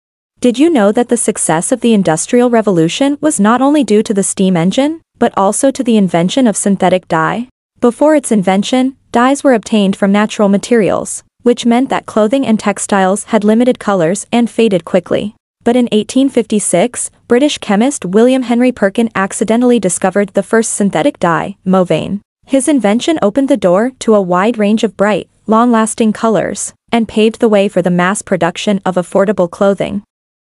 interesting fact #1
Mauvein or malvein, also known as Perkin's purple, mauve, or aniline purple, was the first synthetic organic chemical dye.